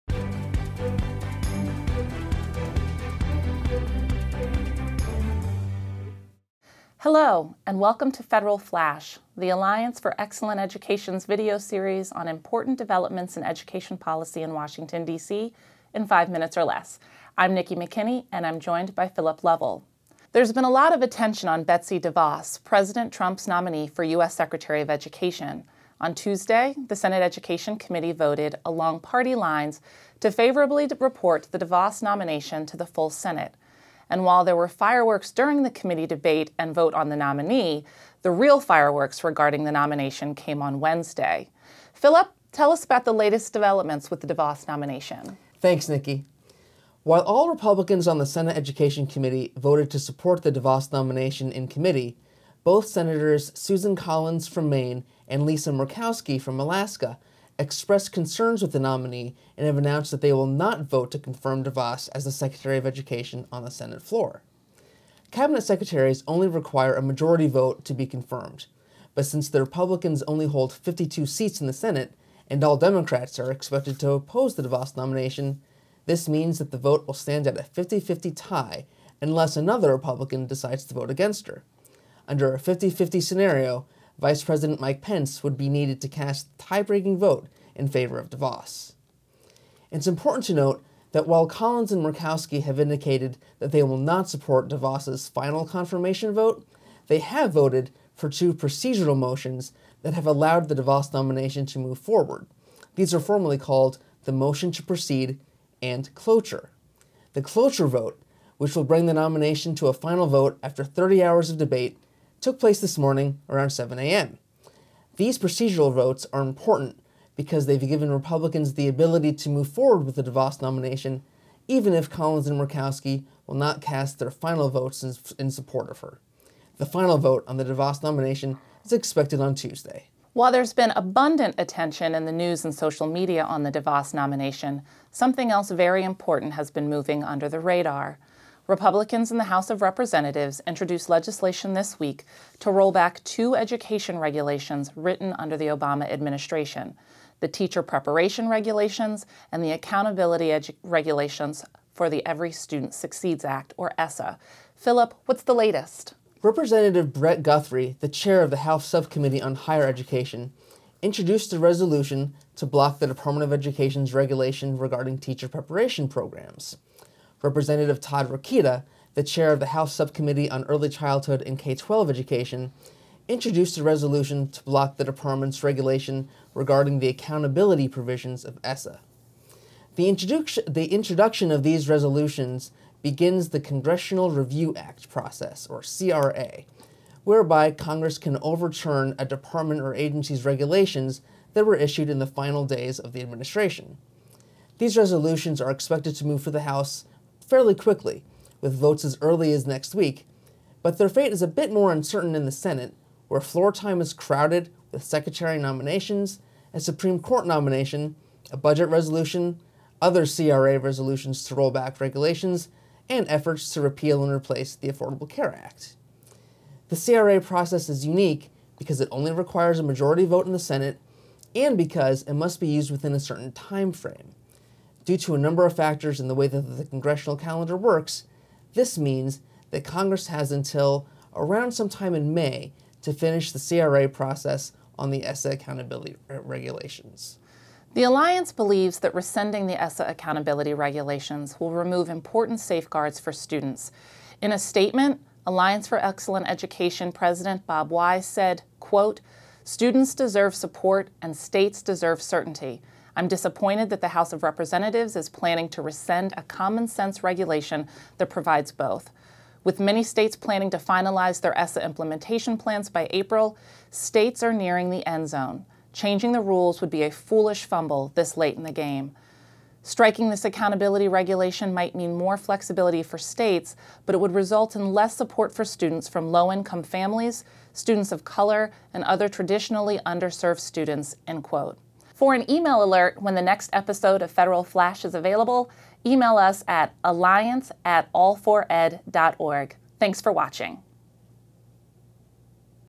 Federal Flash: 2/3/17: The Latest on the DeVos Nomination & ESSA Regulations
While there has been abundant attention on Betsy DeVos, President Trump’s nominee to lead the U.S. Department of Education, something very important has been moving under the radar in the U.S. House of Representatives regarding education regulations written under the Obama Administration. Today’s Federal Flash has the latest on both.